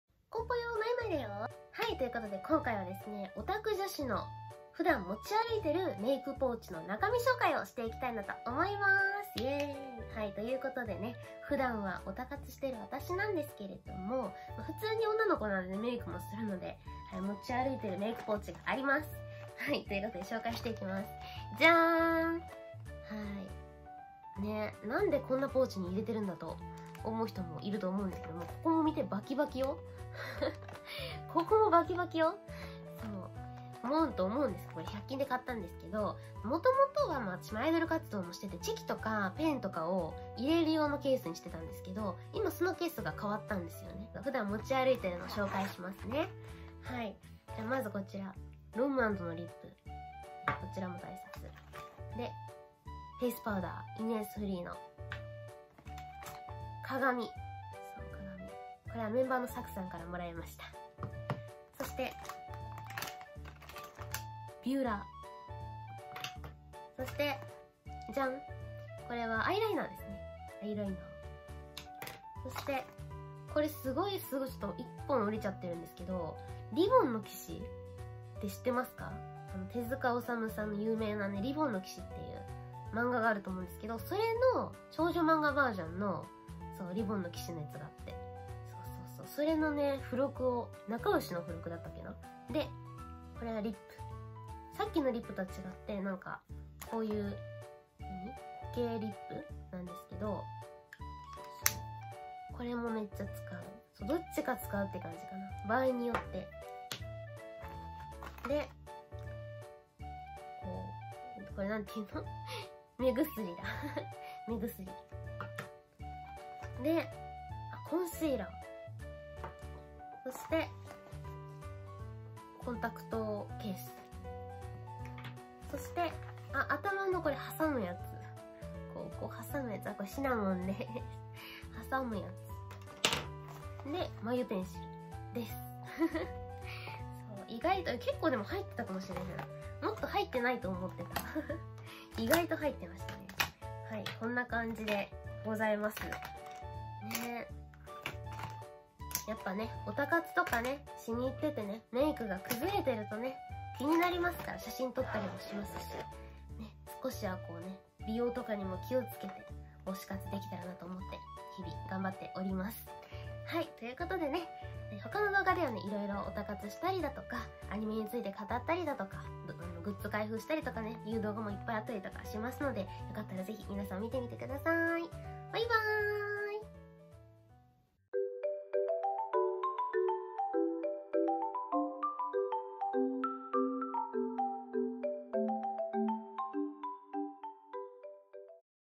What′s in my pouch? /オタク女子のメイクポーチの中身紹介💄✨
普段持ち歩く用のメイクポーチの中身です💗
コンパクト重視！！

ーーーーー

枡川茉衣「まいまい」のSNSも
是非チェックしてね ♪

「枡川茉衣(まいまい)」のLINE LIVEチャンネル